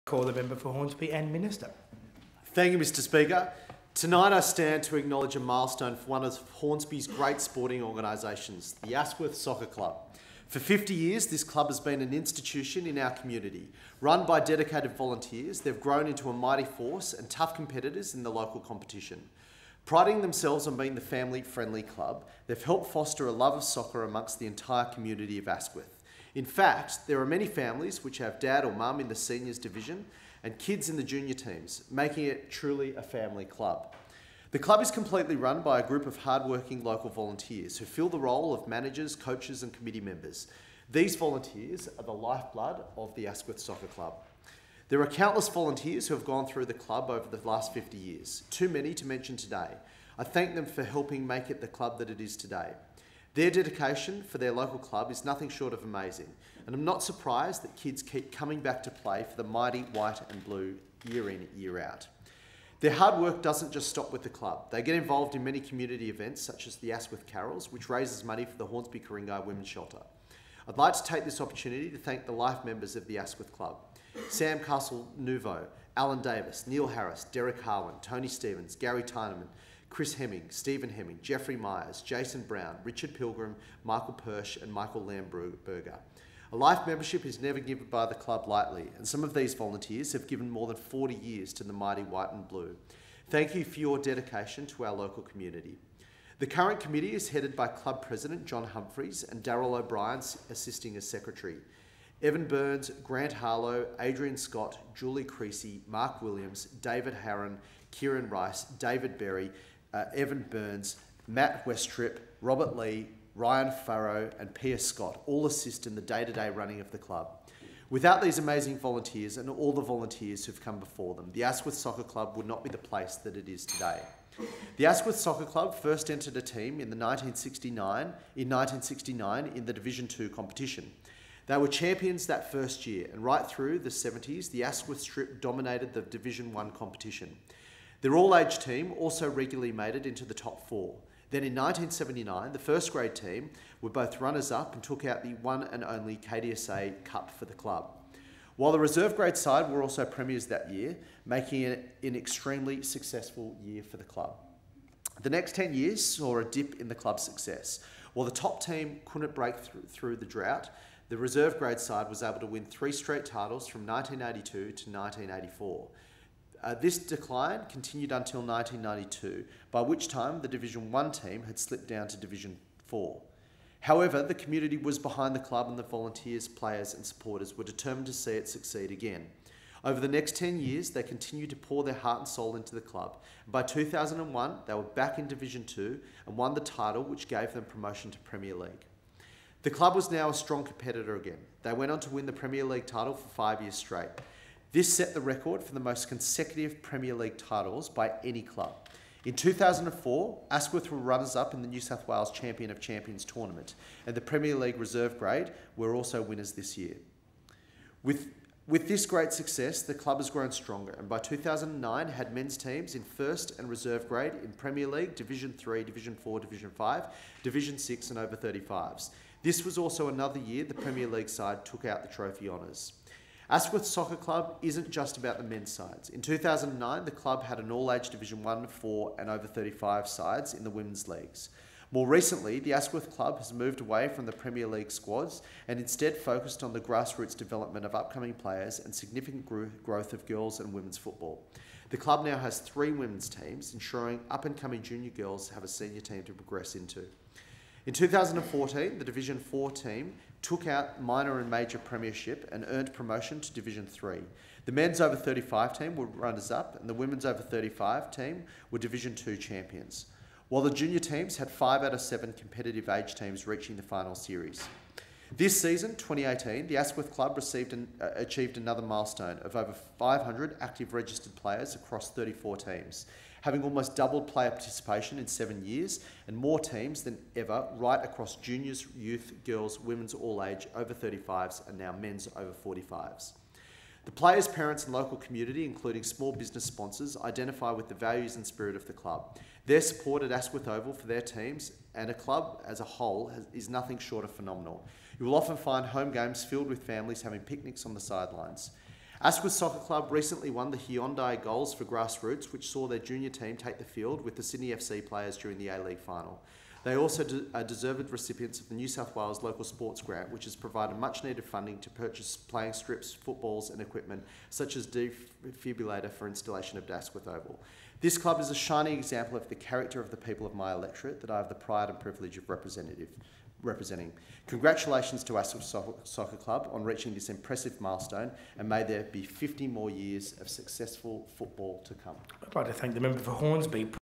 Asquith Soccer Club celebrates 50 years
Minister Kean congratulates Asquith Soccer Club on 50 years.